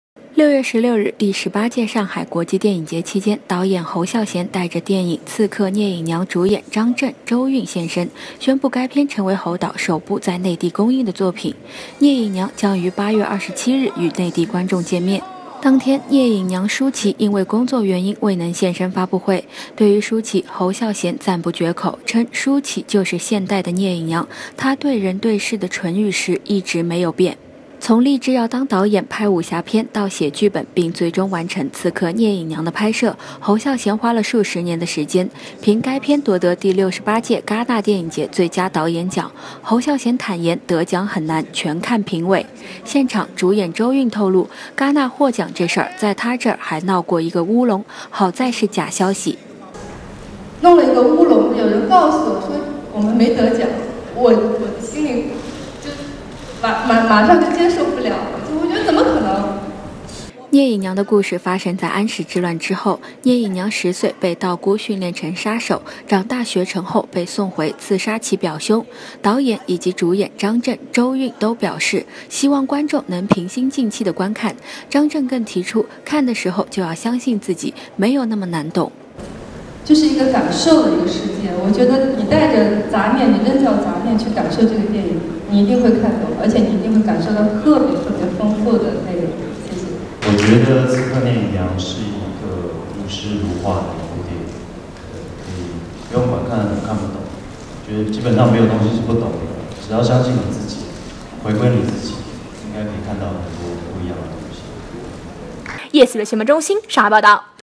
【5年前】侯孝賢贊舒淇就是現代《聶隱娘》 首部內地公映力作定檔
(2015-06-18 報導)

導演侯孝帶著電影《刺客聶隱娘》主演張震、周韻現身，宣佈該片成為侯導首部內地公映的作品，《刺客聶隱娘》將於8月27日與內地觀眾見面。